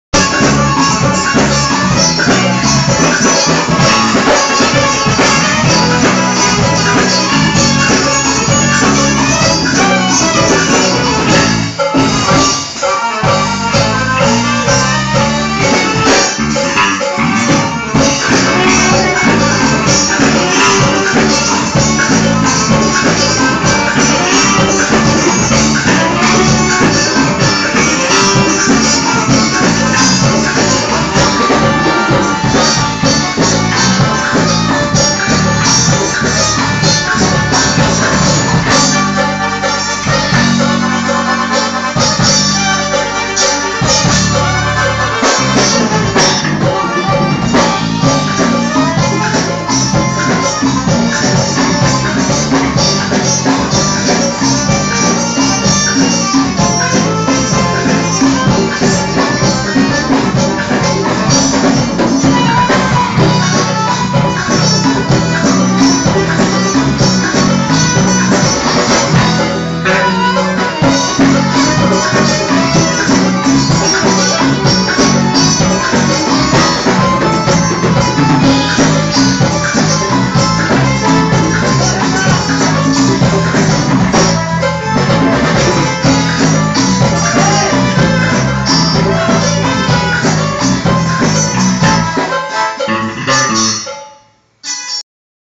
Ensaio de Igor Araújo e Forrozão Sem Limites
Ensaio um dia antes do show em Salgueiro-PE. O sanfoneiro maguinho toca muuito!